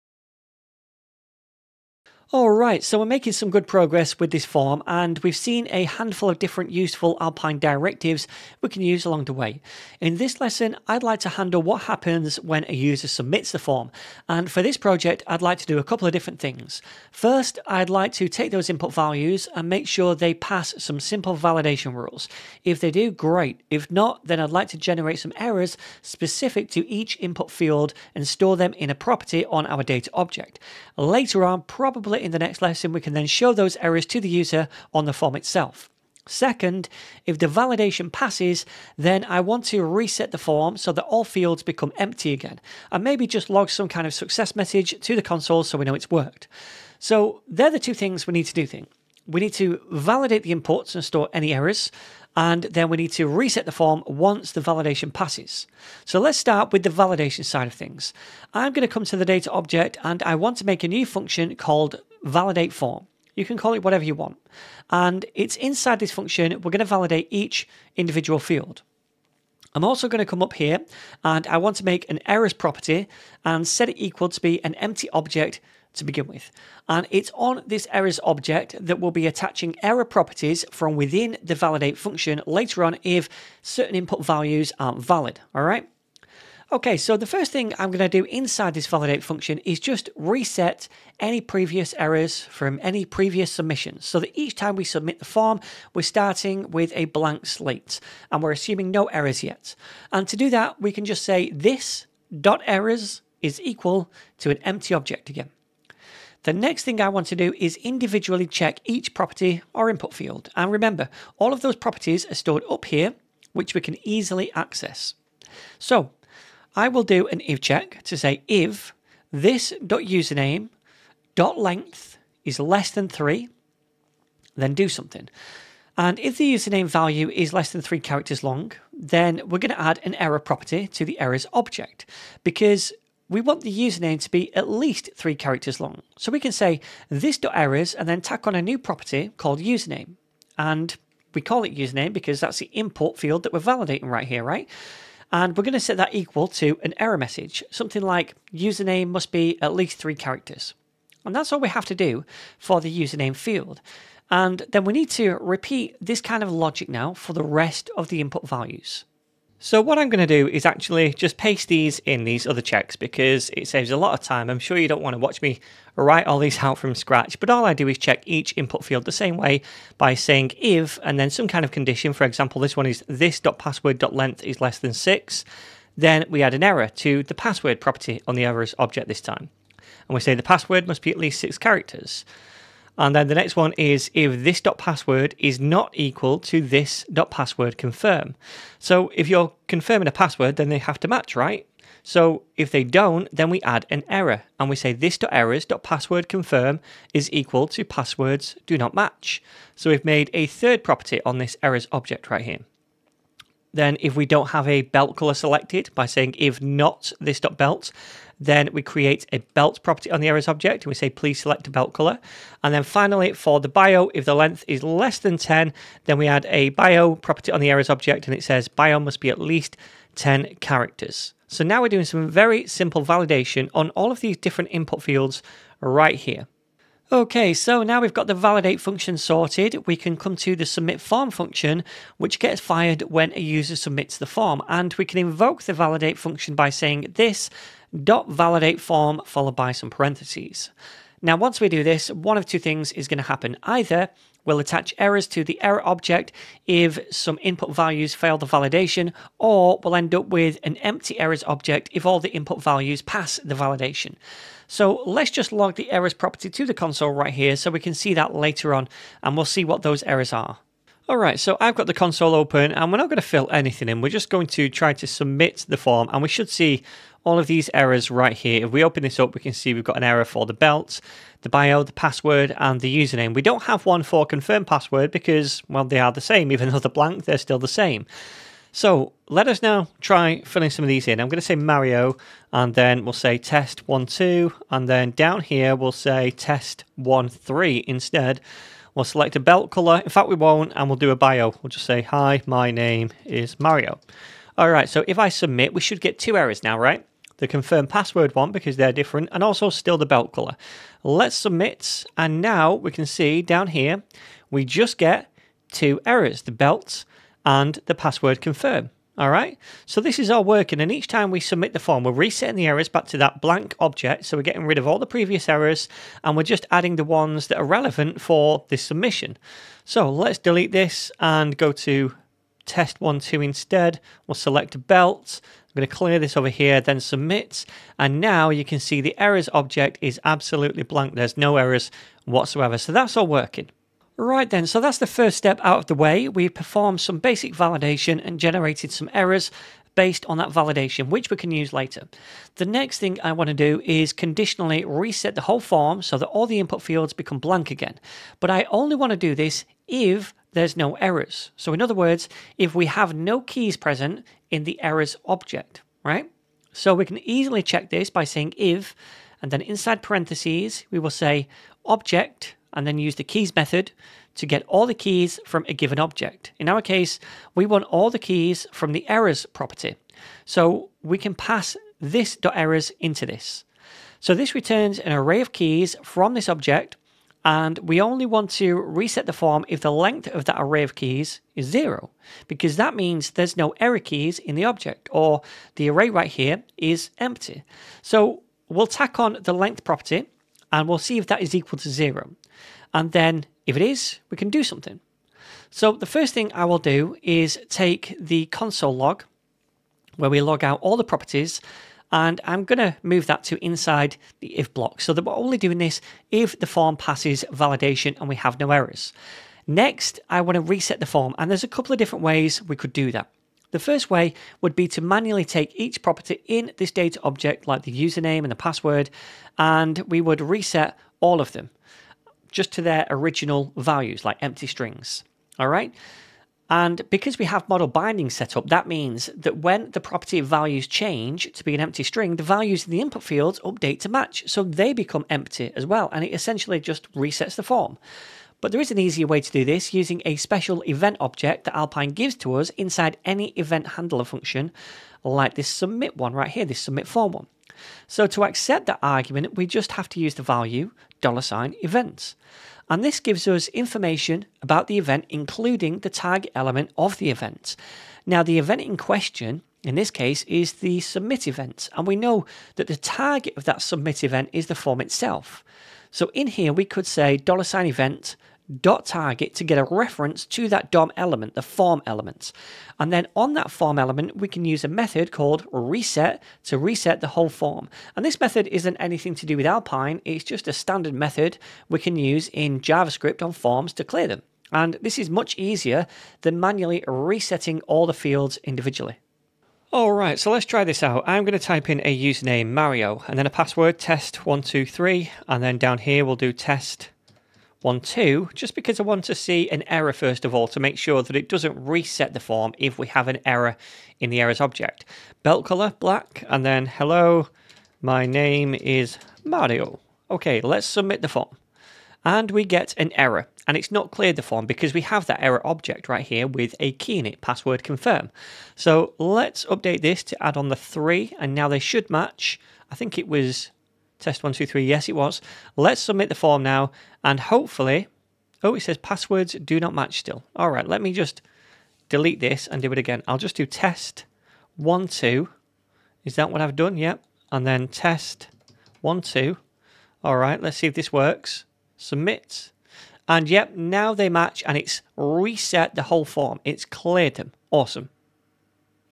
Alpine.js Crash Course #6 - Form Reset & Validation
In this Alpine.js tutorial series, you'll learn what Alpine is, and how we can use it to create dynamic, interactive and reactive user interfaces.